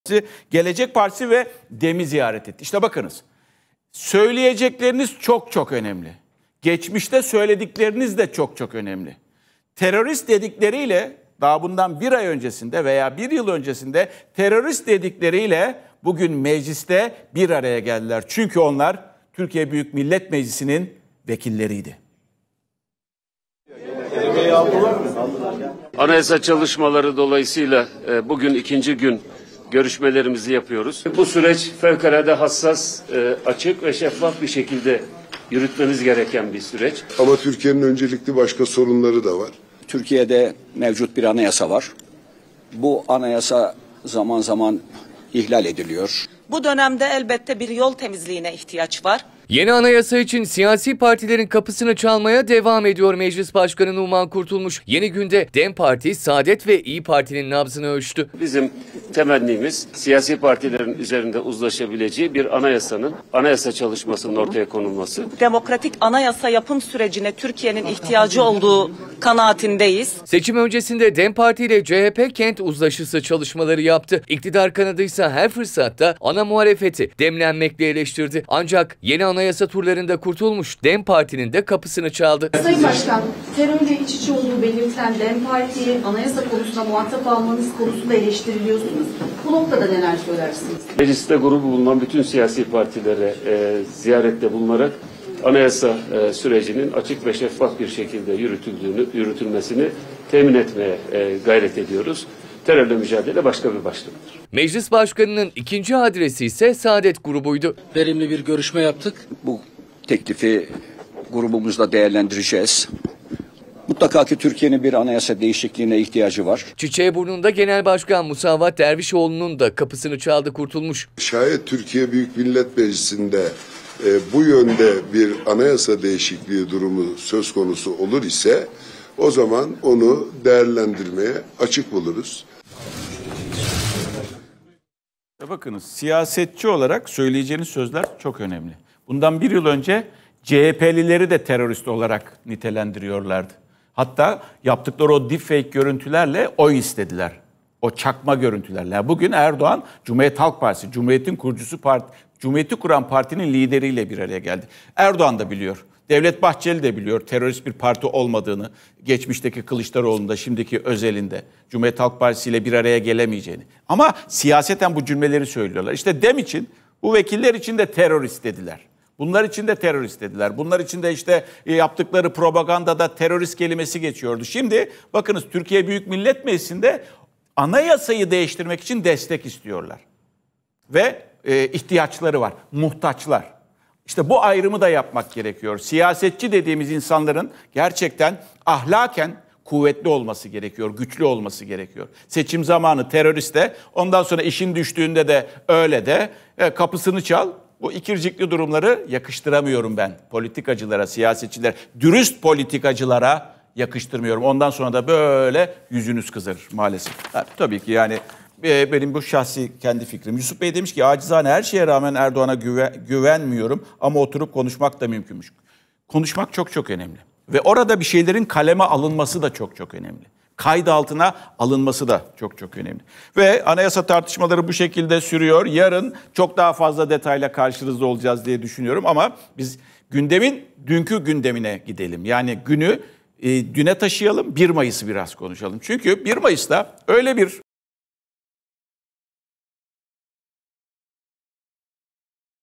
Numan Kurtulmuş DEM Parti'yi Ziyaret Etti! Fatih Portakal O Görüntülere Çok Sinirlendi
Digiturk: 60
D-smart: 93
Turkcell TV Plus: 69
Tivibu: 69
Türksat Kablo TV: 76